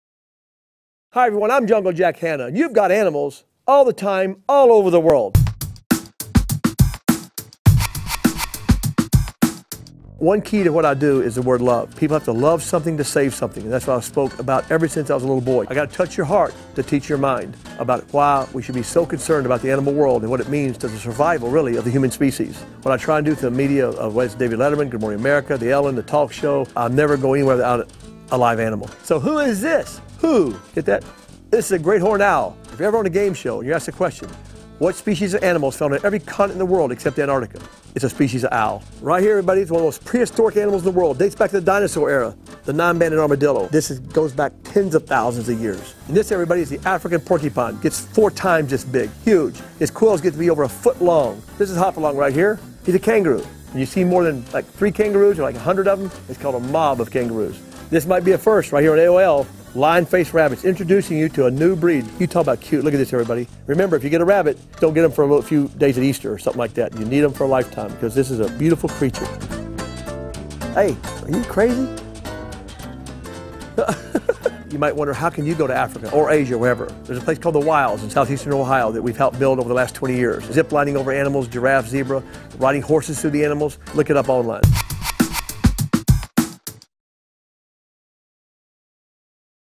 You've Got Jack Hanna
Jack Hanna talks about his passion for animal conservation, and shows off an owl, kangaroo, and porcupine.